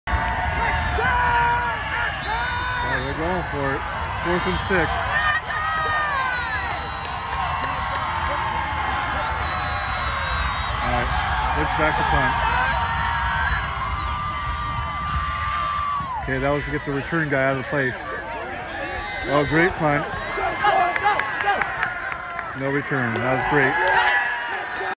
Aptos sets up for run, punts instead
PCAL Gabilan Division football: At Aptos 44, Salinas 41.